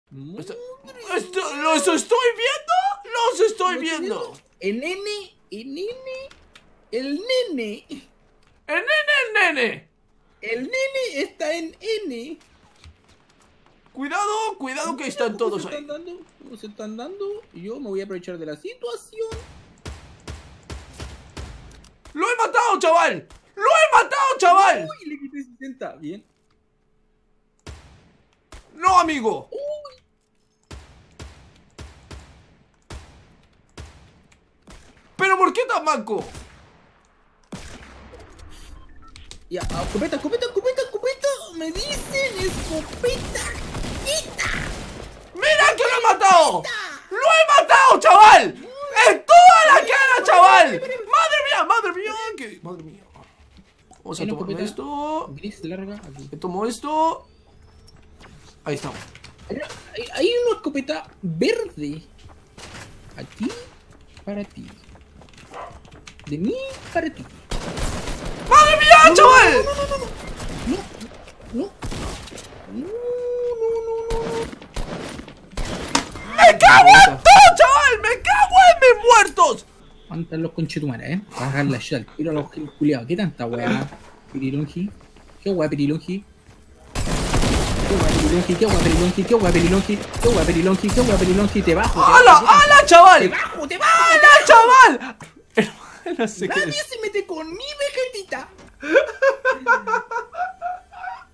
CUANDO 2 VEGETTA777 JUEGAN FORTNITE (PARODIA VEGETTA777)
parodia vegetta777 con mi amigo Mike

 -   D  I  B  U  J  O  S  -

APOYAME CON UNA DONACIÓN PARA INVERTIR EN EL CANAL :D